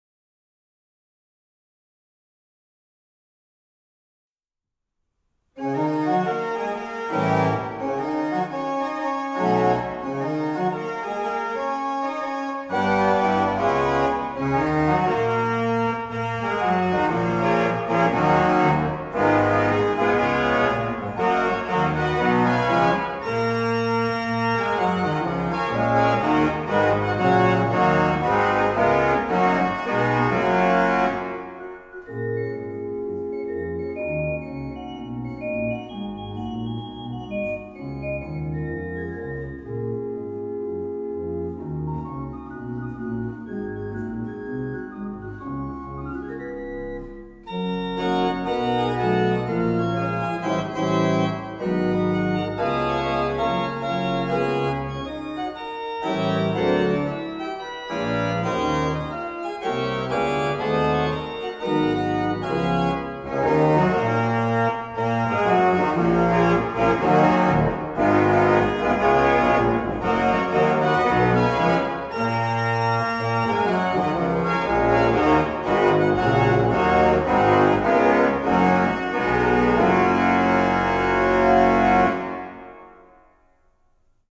Erlebniswelt Orgel - Orgel goes Big Band
Die einzelnen Register und Registerkombinationen der historischen Christian Vater Orgel, die in der St. Nicolai-Kirche in Gifhorn zu hören und zu erleben ist, werden von Kantor Raphael Nigbur klanglich veranschaulicht.